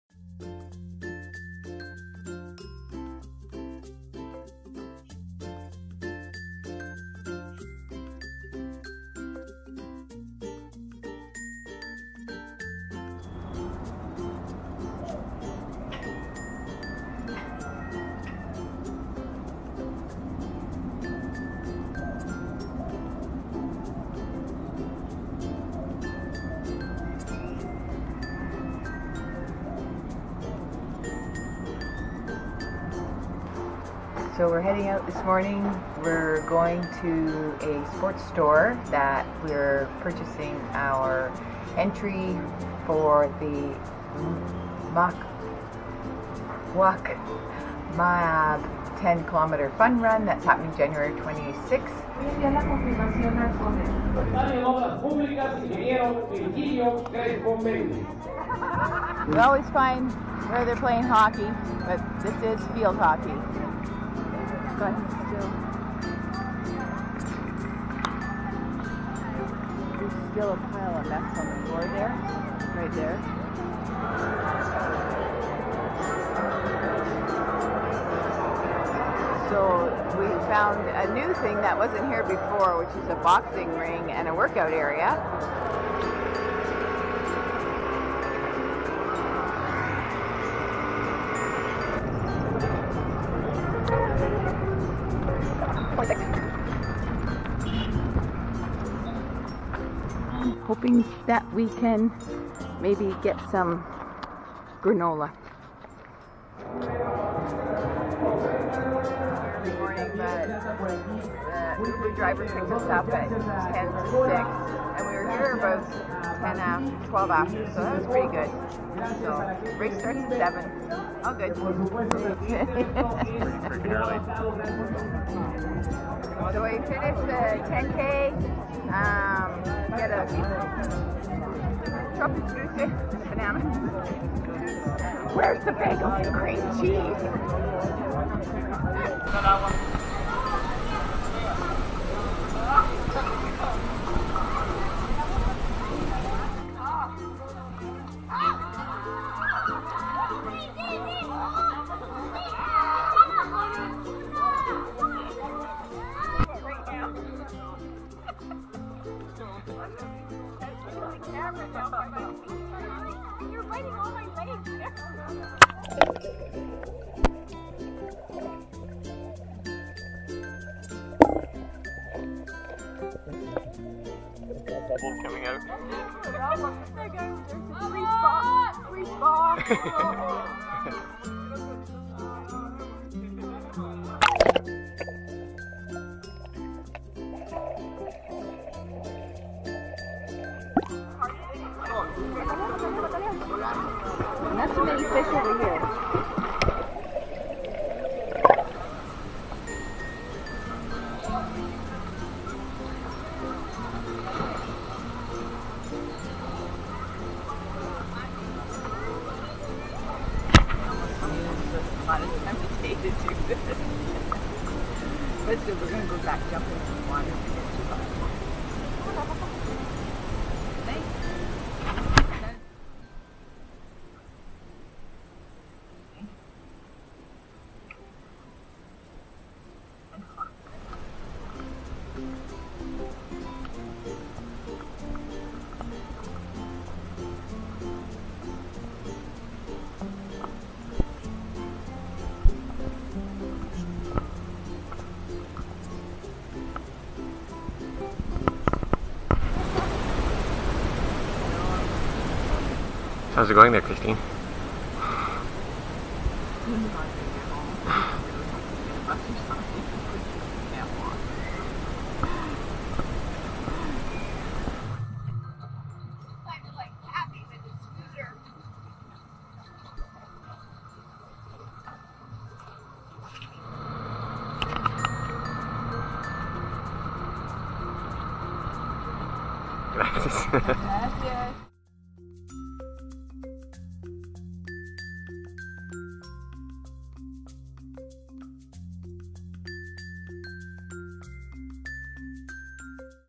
Mexico January 2020
Some of the highlights from our first month here. Seems like everyday is an amazing day. hope you all enjoy!